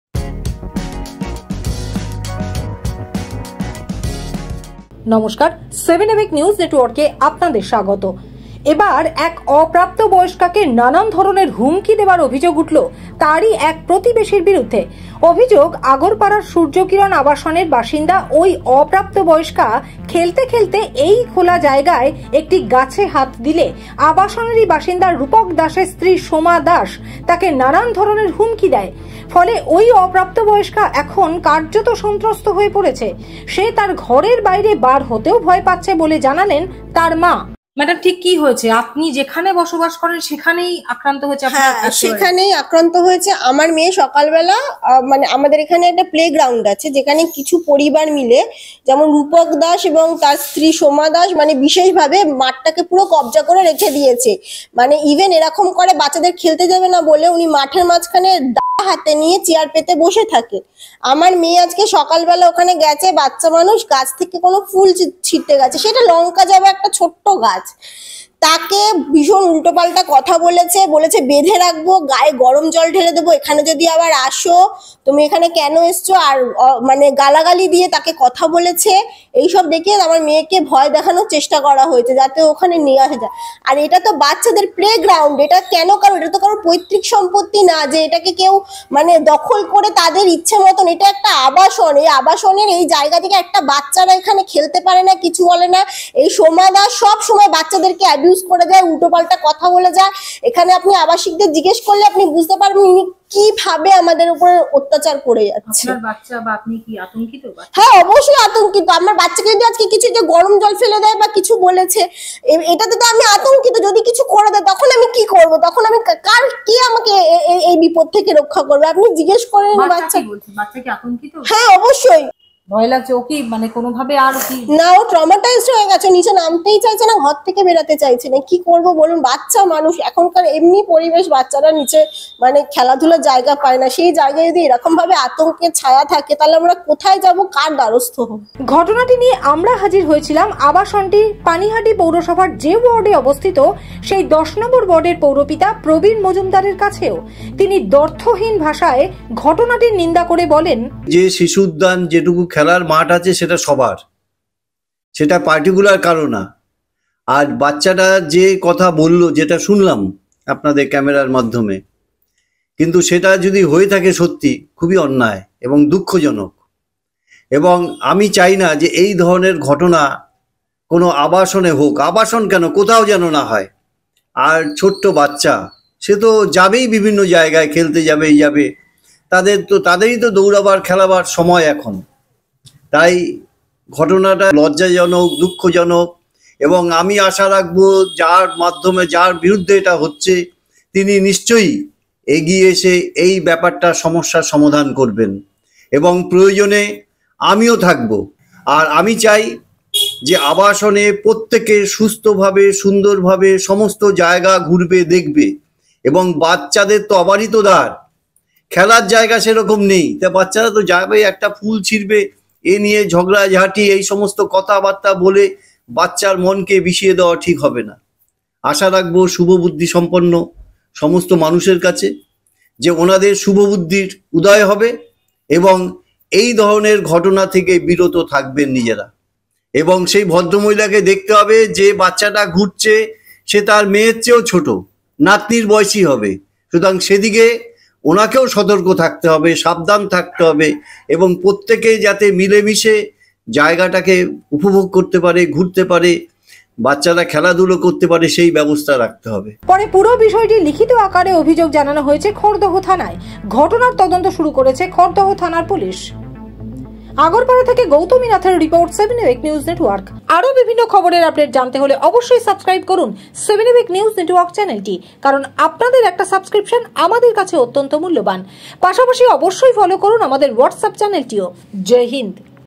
গাছে হাত দেওয়ার অপরাধে ১ অপ্রাপ্তবয়স্কাকে হুমকী,অভিযোগ প্রতিবেশীর বিরুদ্ধে, চাঞ্চল্য আগরপাড়ায়
Jai HInd!